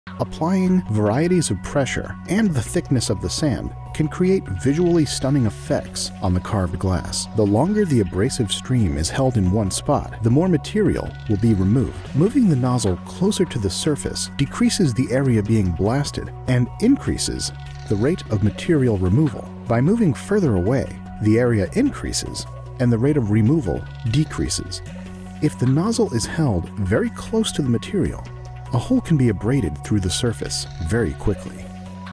Palace of Glass Short Video Series: Deep Carved Glass, Part 4
An alternate etch, known as a negative etch, can be achieved by leaving the resist on the pattern and removing the background areas. Functionally, that allows for the back surface of the etched glass to remain smooth. Can be useful for creating glass murals and backsplashes, that can be mounted on stand-offs or glued with adhesive silicons, used for installation of mirrors.
Different techniques of etching over decorative foil, can be added for creativity.

For more decorative glass styles please visit our official web